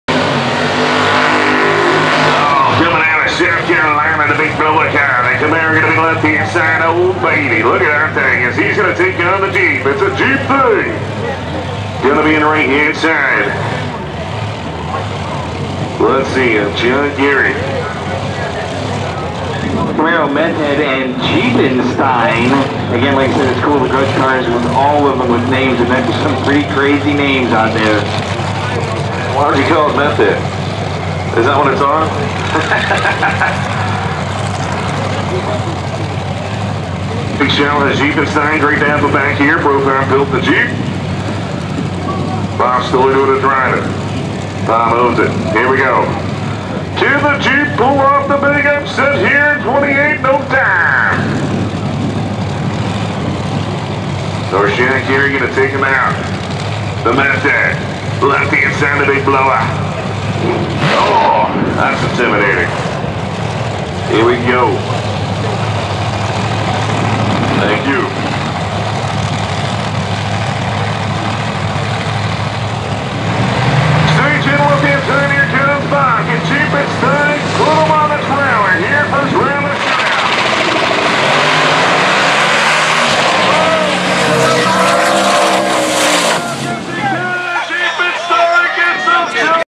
JEEPENSTEIN BEATS SUPERCHARGED CAMARO @ WORLD STREET NATIONALS 26
ORLANDO SPEED WORLD DRAGWAY - Camera : Canon VIXIA HF R80